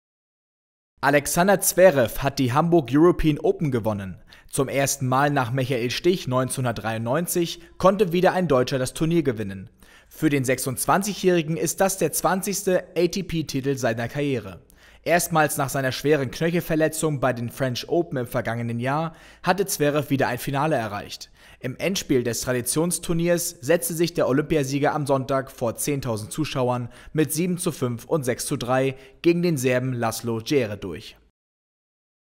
Alexander Zverev gewinnt Hamburg European Open
Alexander Zverev hat die Hamburg European Open gewonnen. Zum ersten mal nach Michael Stich 1993 konnte wieder ein deutscher das Turnier gewinnen. Für den 26-Jährigen ist das der 20. ATP-Titel seiner Karriere. Erstmals nach seiner schweren Knöchelverletzung bei den French Open im vergangenen Jahr hatte Zverev wieder ein Finale Erreicht. Im Endspiel des Traditionsturniers setzte sich der Olympiasieger am Sonntag, vor 10.000 Zuschauern, mit 7:5 und 6:3 gegen den Serben Laslo Djere durch.